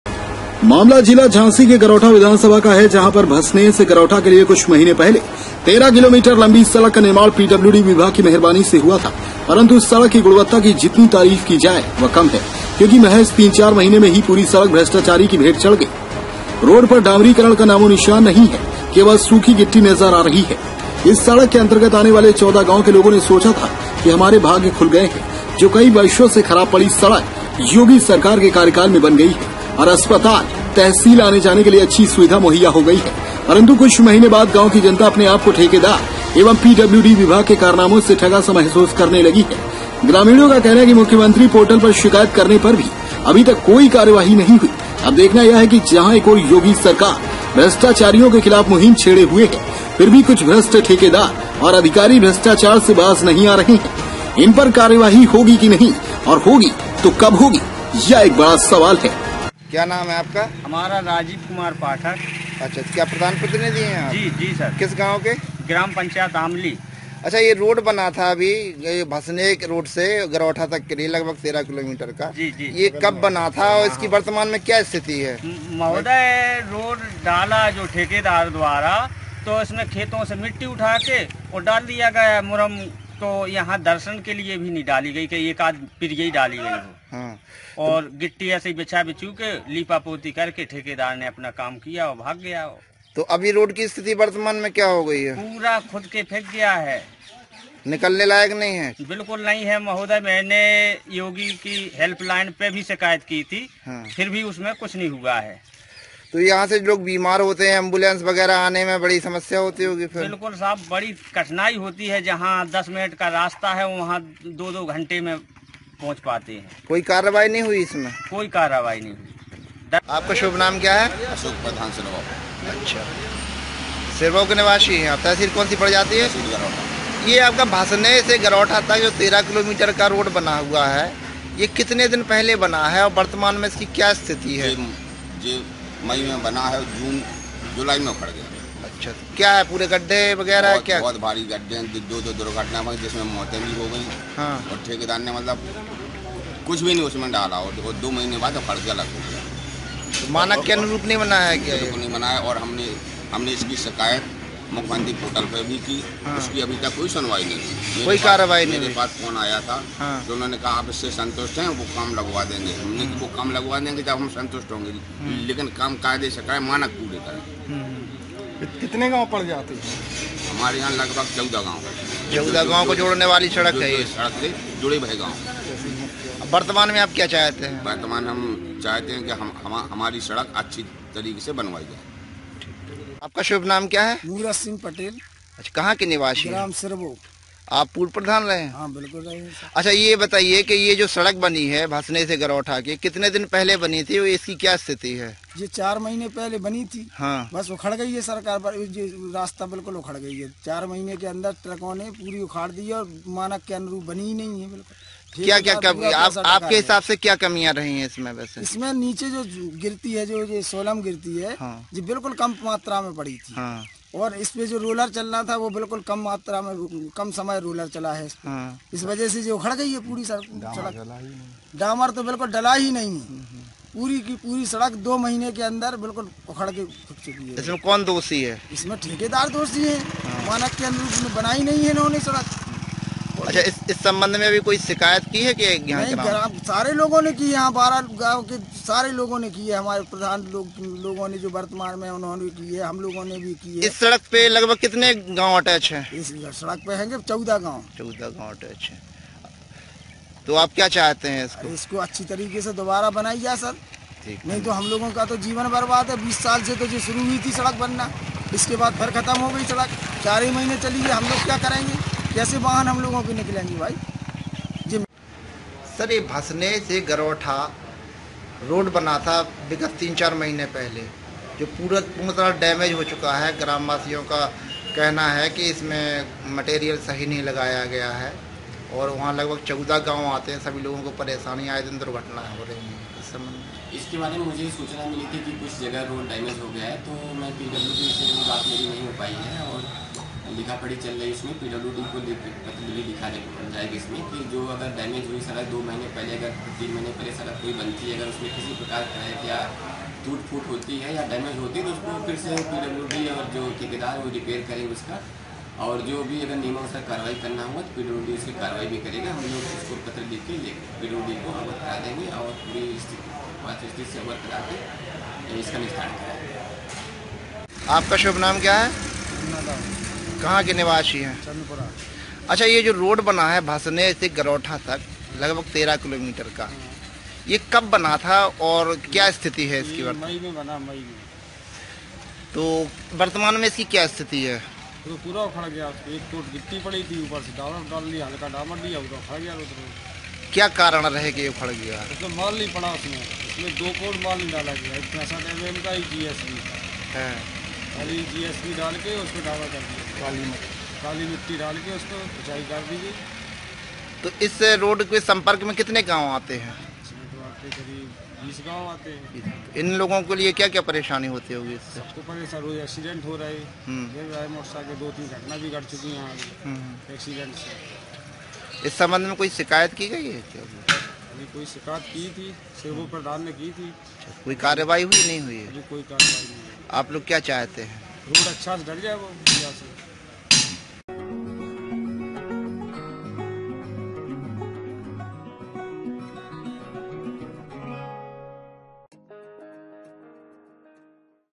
। Nonstop News । झांसी मैं सड़क निर्माण में बड़ा घोटाला 13 किलोमीटर लंबी सड़क नहीं चली 13 महीने ।
लोकेशन -झांसी
रिपोर्टर- विनोद सोनी
दिनाँक- 11/10/21

स्लग - झांसी मैं सड़क निर्माण में बड़ा घोटाला 13 किलोमीटर लंबी सड़क नहीं चली 13 महीने

एकर- मामला जिला झांसी के गरौठा विधानसभा का है जहां पर भसनेह से गरौठा के लिए कुछ महीने पहले 13 किलोमीटर लंबी सड़क का निर्माण पीडब्ल्यूडी विभाग की मेहरबानी से हुआ था परंतु सड़क की गुणवत्ता की जितनी तारीफ की जाए वह कम है क्योंकि कि महज तीन चार महीने में ही पूरी सड़क भ्रष्टाचारी की भेंट चढ़ गई रोड पर डामरीकरण का नामोनिशान नहीं है केवल सूखी गिट्टी नजर आ रही है इस सड़क के अंतर्गत आने वाले 14 गांव के लोगों ने सोचा था कि हमारे भाग्य खुल गए हैं जो कई वर्षों से खराब पड़ी सड़क योगी सरकार के कार्यकाल में बन गई है और अस्पताल, तहसील आने जाने के लिए अच्छी सुविधा मुहैया हो गई है परंतु कुछ महीने बाद गांव की जनता अपने आप को ठेकेदार एवं पीडब्ल्यूडी विभाग के कारनामों से ठगा सा महसूस करने लगी हैं ग्रामीणों का कहना है कि मुख्यमंत्री पोर्टल पर शिकायत करने पर भी अभी तक कोई कार्यवाही नहीं हुई अब देखना यह है कि जहां एक और योगी सरकार भ्रष्टाचारियों के खिलाफ मुहिम छेड़े हुए हैं फिर भी कुछ भ्रष्ट ठेकेदार और अधिकारी भ्रष्टाचार से बाज नहीं आ रहे है इन पर कार्यवाही होगी कि नहीं और होगी तो कब होगी यह एक बड़ा सवाल है

 बाईट -ग्रामीणों की

 बाईट -धीरेंद्र प्रताप सिंह एसडीएम